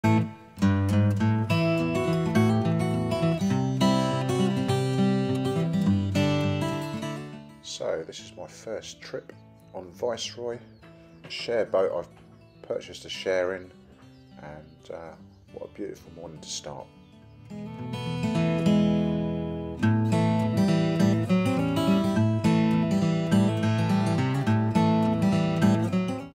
001 Oxford Canal near Braunston March 2022 SD 480p
One lovely early spring morning on the Oxford, around 8am. Lovely mist.